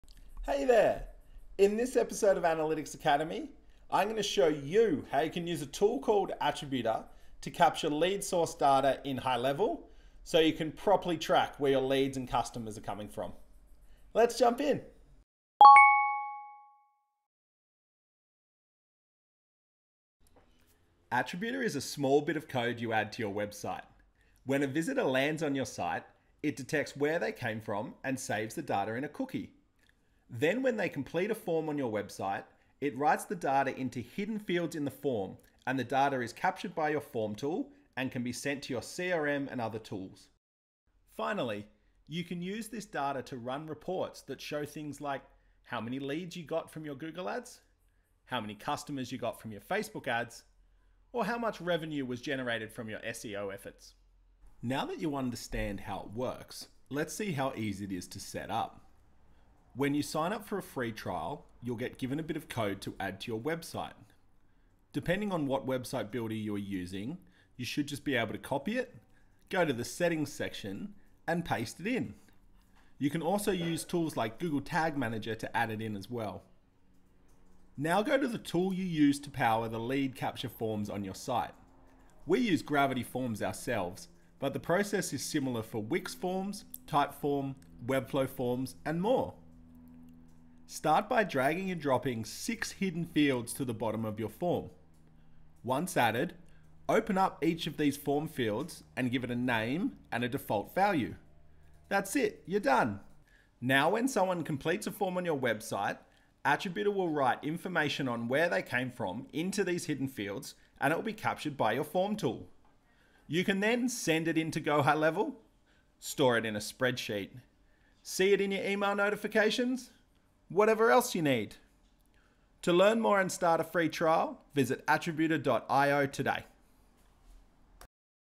Track Lead Source in GoHighLevel
Learn how to capture information on the source of your leads in GoHighLevel, so that you can track which channels & campaigns (I.e. Google Ads, Facebook Ads, SEO, etc) are generating the most leads & customers for your business.